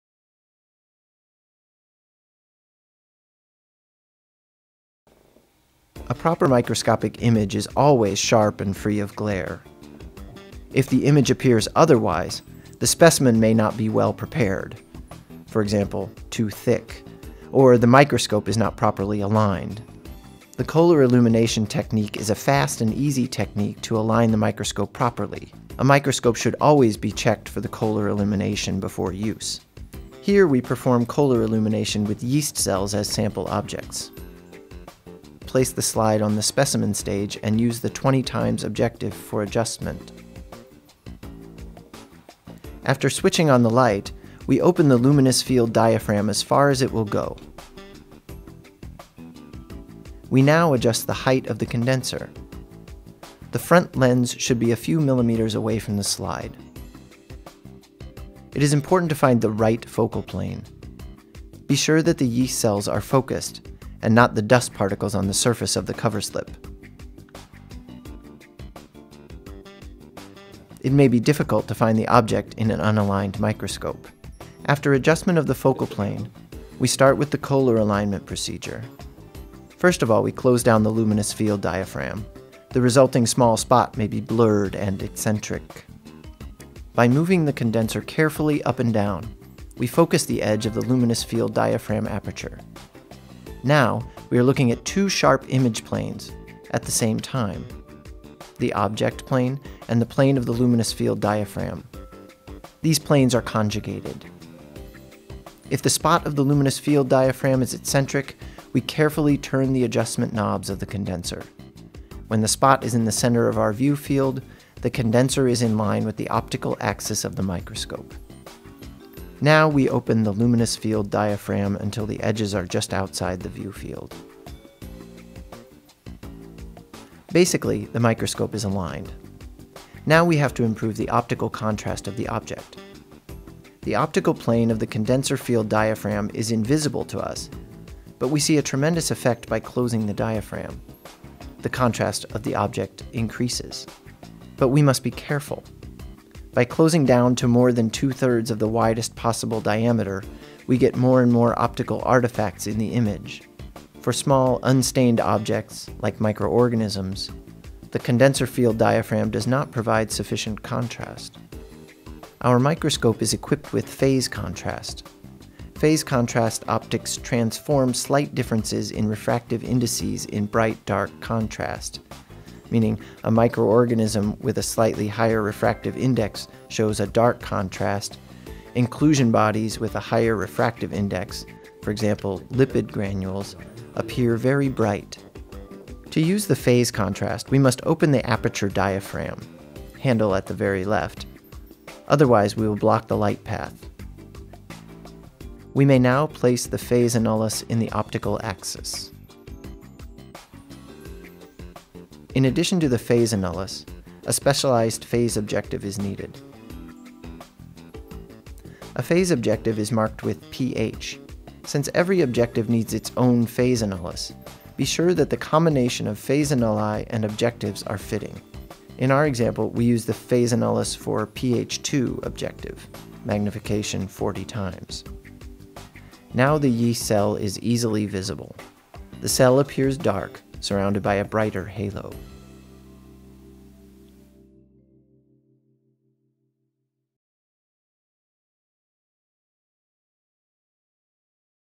Koehler Illumination Microscope
In this video you learn how to operate a microscope to get a brilliant picture.